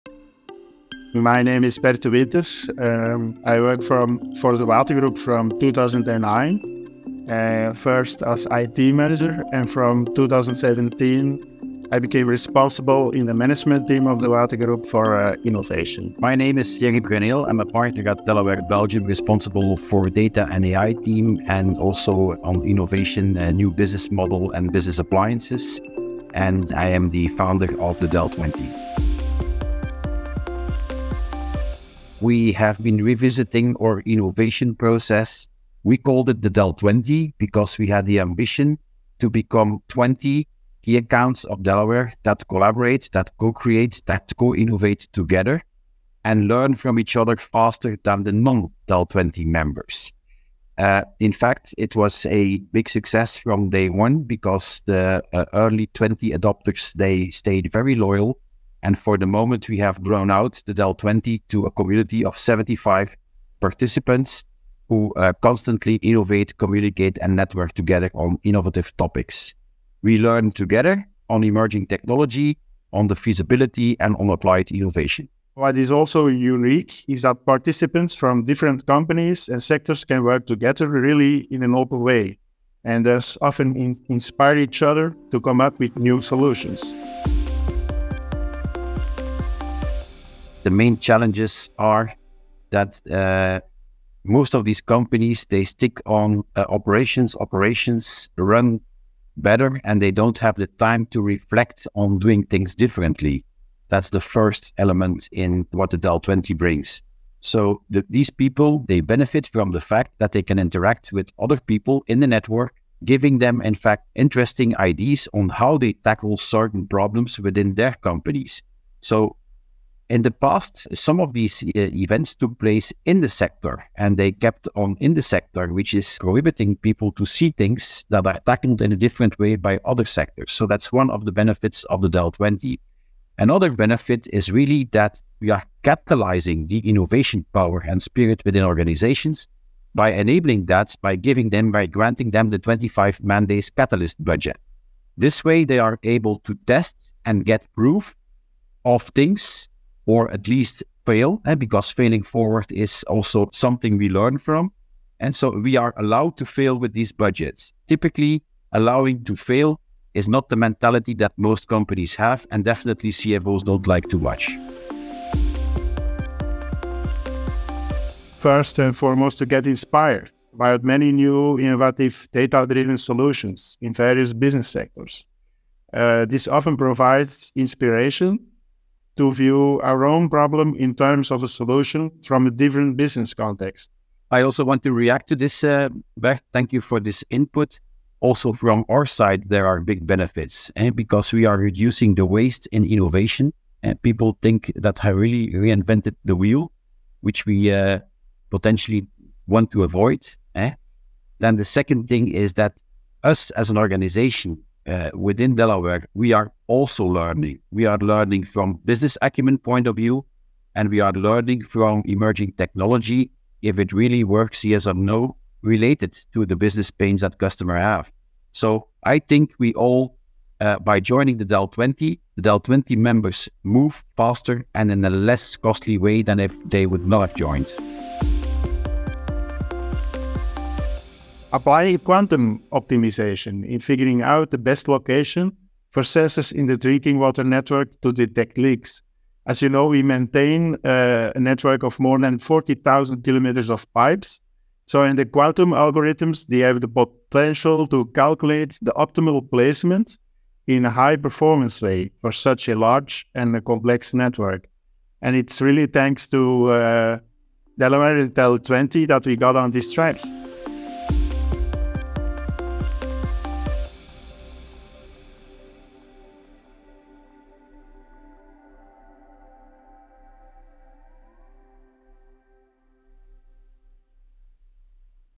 2018 - Le pouvoir d'apprendre les uns les autres
Pour atteindre 150 ans, il faut oser se remettre en question et se réinventer.

Bert De Winter, Responsable de l'équipe Innovation de De Watergroep et Thierry Bruyneel, fondateur du DEL20, associé chez delaware et responsable de l'équipe Data et IA discutent des avantages de rejoindre la communauté du DEL20 et de l'importance de travailler avec d'autres entreprises pour s'inspirer mutuellement et trouver de nouvelles solutions plus performantes.

"Les participants bénéficient d’interactions avec d'autres membres du DEL20, ce qui leur donne des idées intéressantes sur la façon dont ils abordent certains problèmes dans leurs entreprises" - Bert De Winter, Responsable de l'équipe Innovation de De Watergroep